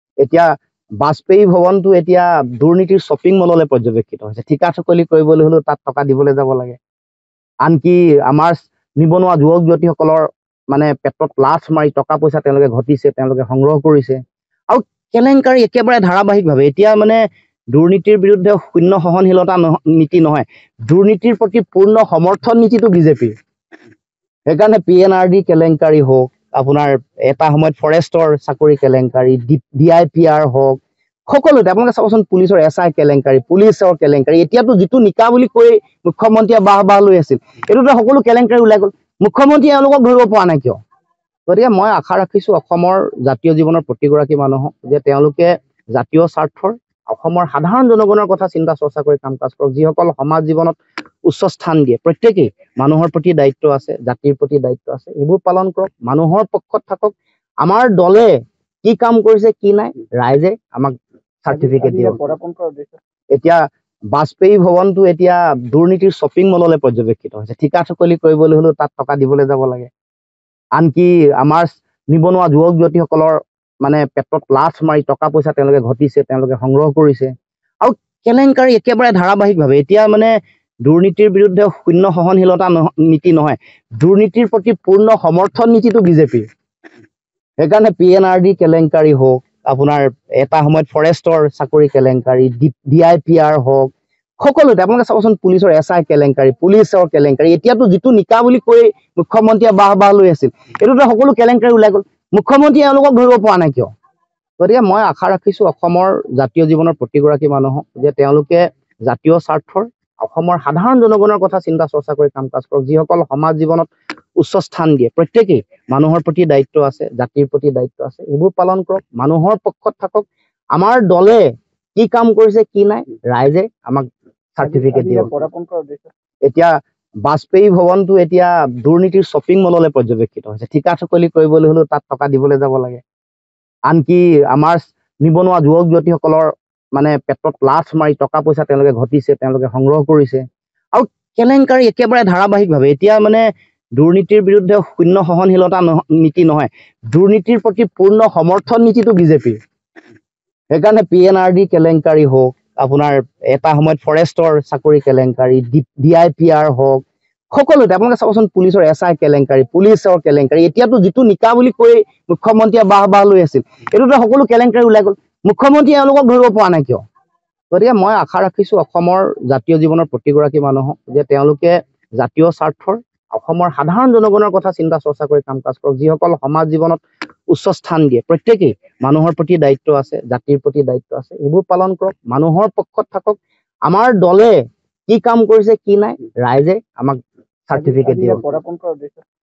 Lurin Jyoti Gogoi News : নিৱনুৱাৰ পেটত লাট মাৰি টকা সংগ্ৰহ কৰিছে বিজেপিয়ে: লুৰিণ জ্যোতি গগৈ | N18V
Lurin Jyoti Gogoi News |  বিজেপিক সমালোচনা Lurin Jyoti Gogoi ৰ ।  নিৱনুৱাৰ পেটত লাট মাৰি টকা সংগ্ৰহ কৰিছে বিজেপিয়ে: লুৰিণ জ্যোতি গগৈ